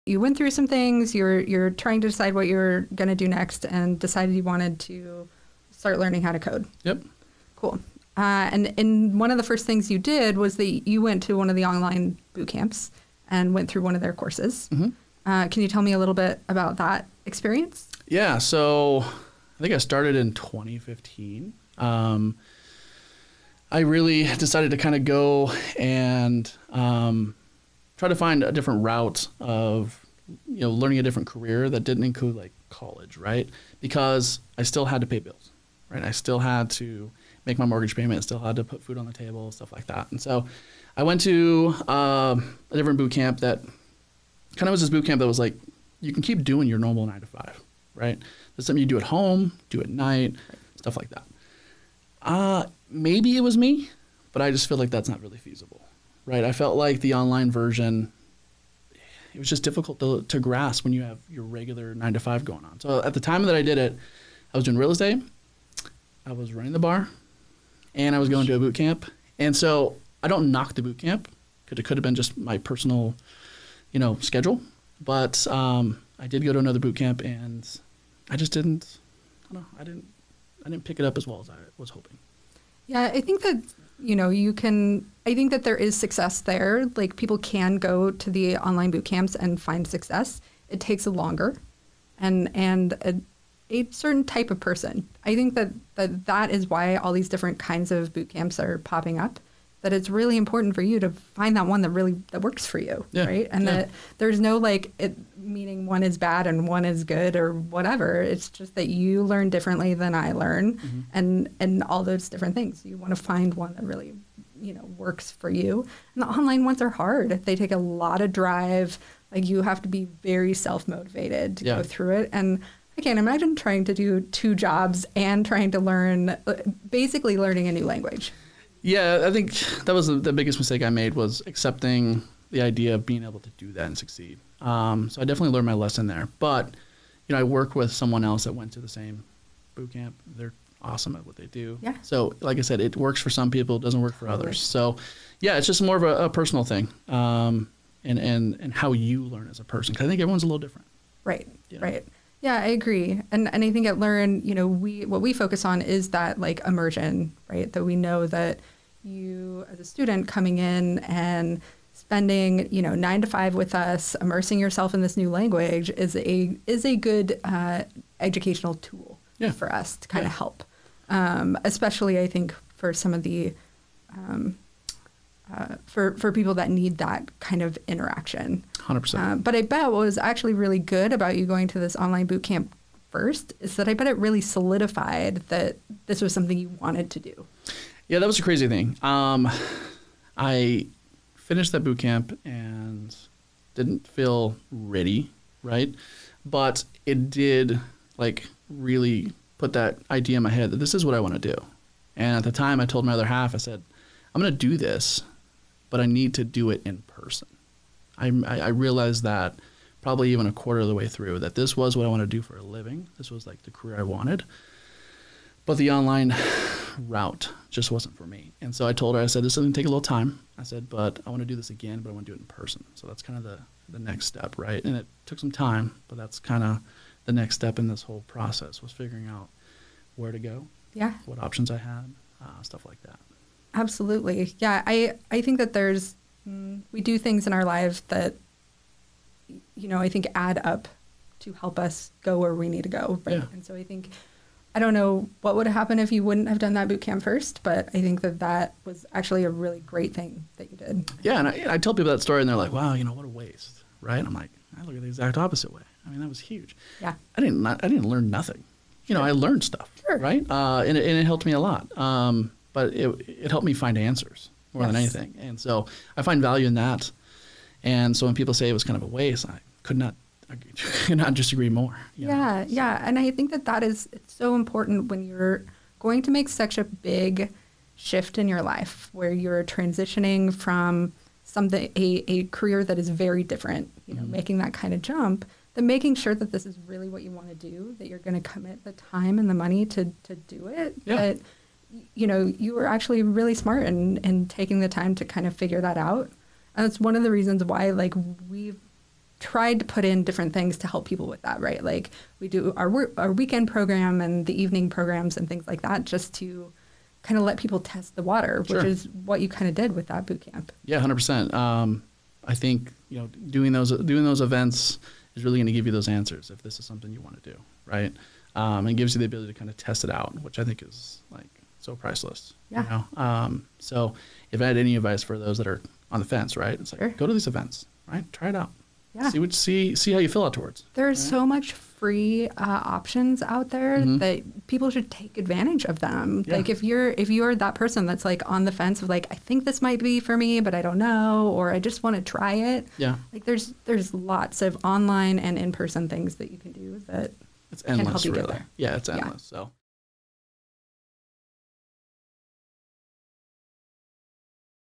Coding Bootcamps: Online vs In-Person
When looking into coding bootcamps one of the biggest decisions you will have to make is will you attend an online coding bootcamp or an in-person coding bootcamp? Each have their own advantages and we talk with one of our alumni Tucker Mullen who went through an online program before he attended LEARN academy. He shares some of the biggest differences between the two.

Taken From Collaboration Code Radio Episode 03 - Going All-In w/ Tucker Mullen.